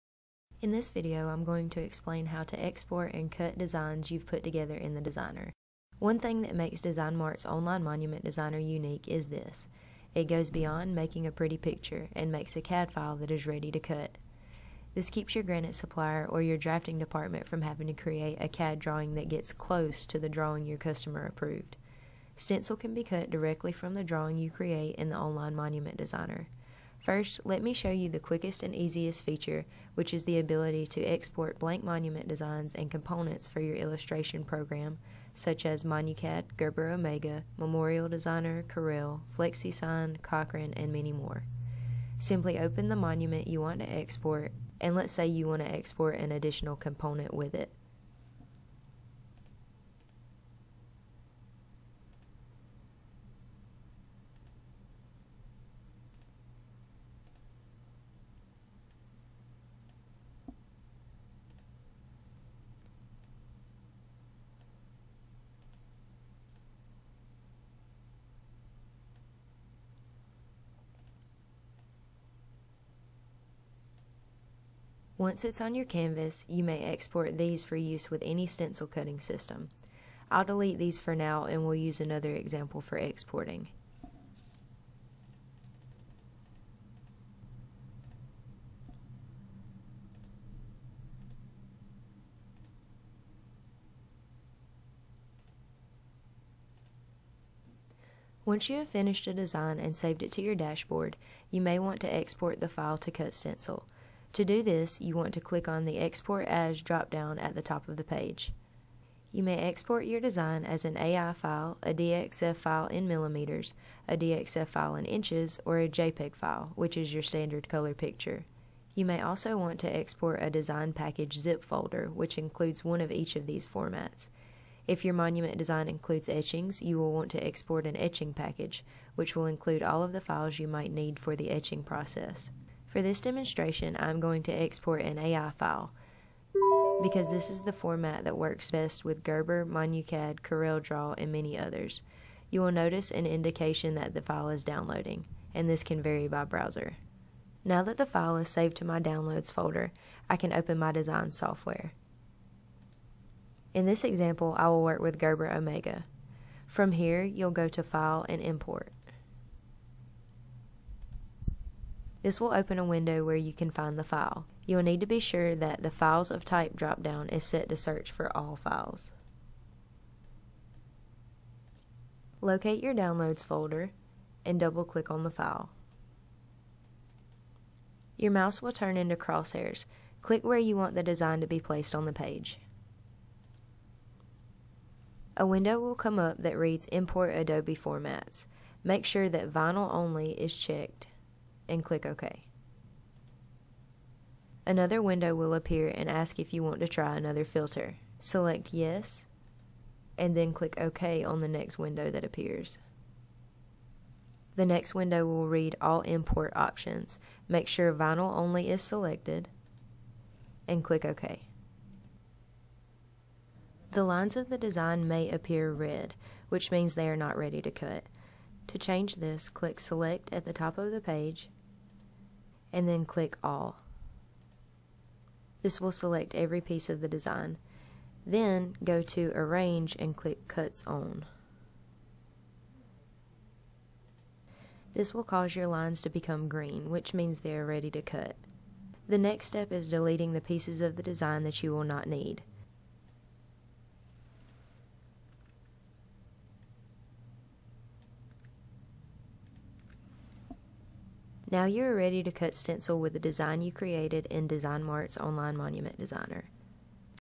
Export & Cut Your Work for Gerber
In this tutorial video, I'll explain how to export your finished designs to Gerber Omega as CAD file that you may cut stencil with. We'll also cover how to set up the file in your CAD program to get it ready to cut. We can connect to you remotely and share the mouse as we answer your questions.  Other videos will cover the additional capabilities available in the Online Monument Designer.